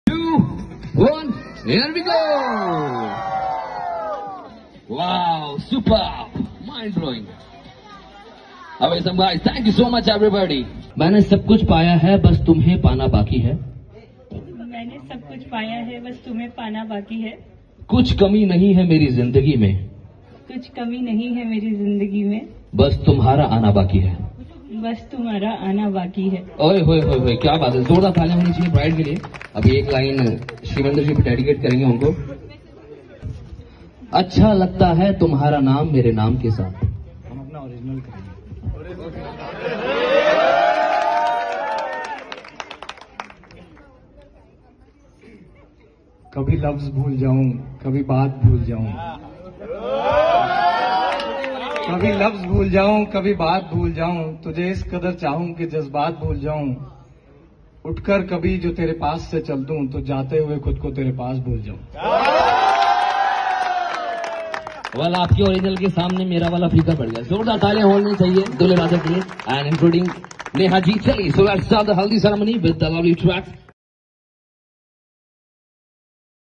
Jab Dulhe Raja ne Mari Baji to Dulhan Bhi Sharma Gyi | Best Anchor in Jaipur
Video Source: Anchor Garv Dhingra  -  Jaipur